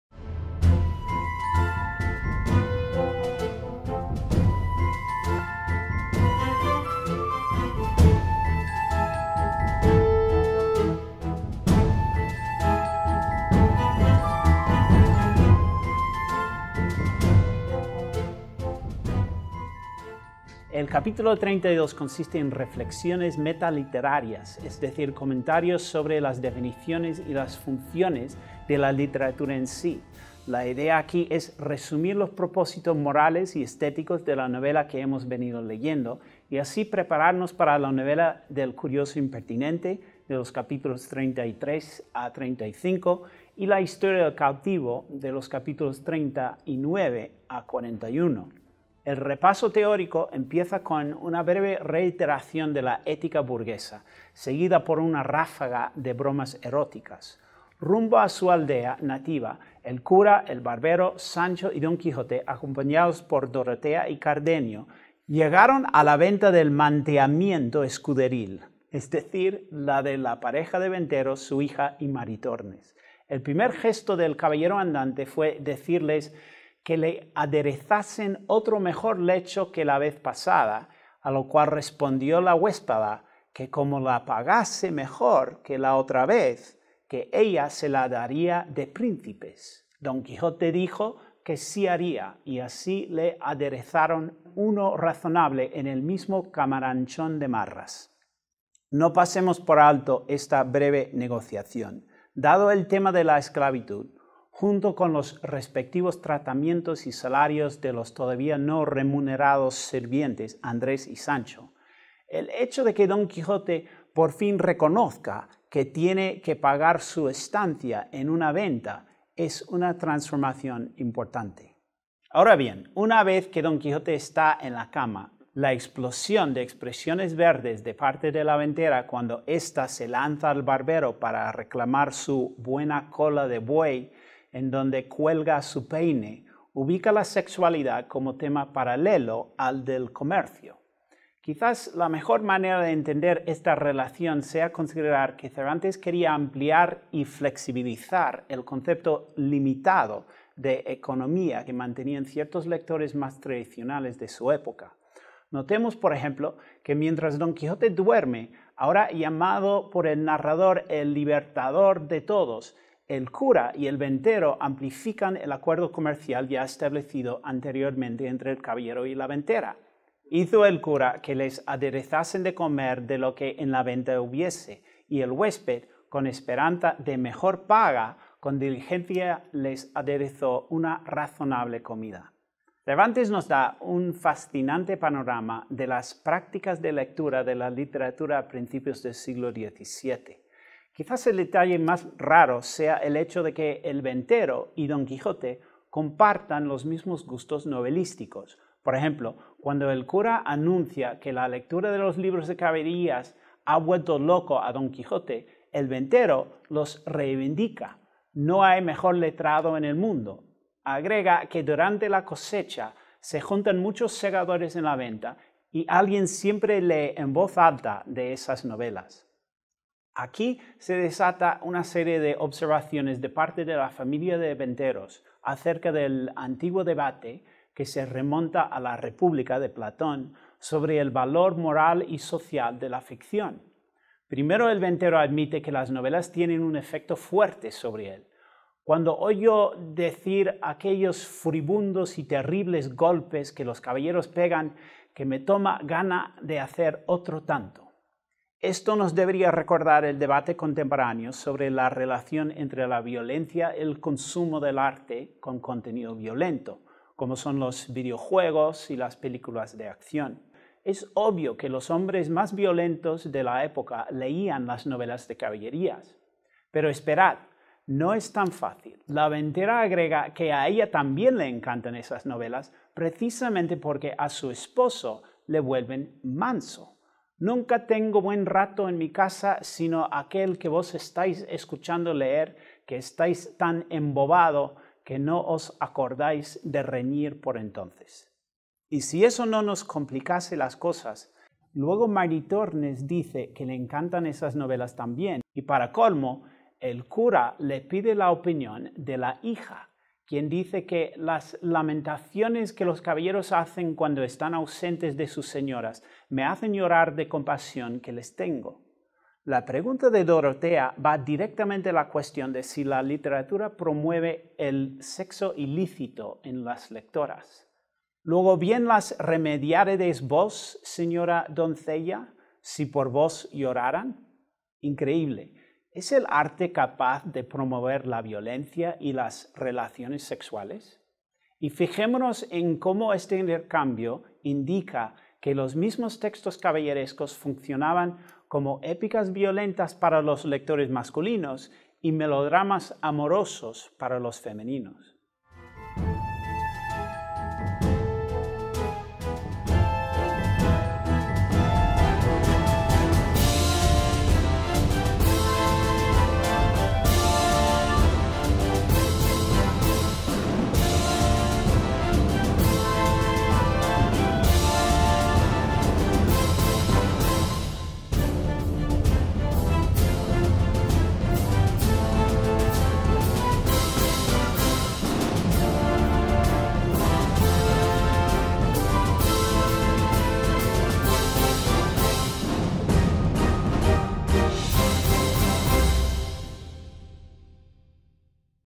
Lección 6 El encantamiento de las novelas de caballerías
Lección 6 - Módulo 3. En este vídeo se explica el capítulo 32 del libro “Don Quijote de la Mancha” de Miguel de Cervantes.

El curso tiene como objetivo presentar la mejor novela en español, a través de una serie de vídeos cortos y recursos digitales. En el Módulo 3 el profesor Eric C. Graf explica los capítulos 29 al 52 del libro.

Eric C. Graf es catedrático de literatura en la Universidad Francisco Marroquín. Se doctoró en literatura española en la Universidad de Virginia, sus áreas de especialización incluyen: literatura española medieval y moderna, filosofía renacentista, historia de la novela y teoría literaria, política, cultural y económica.

Créditos:
Una producción de New Media | UFM 2015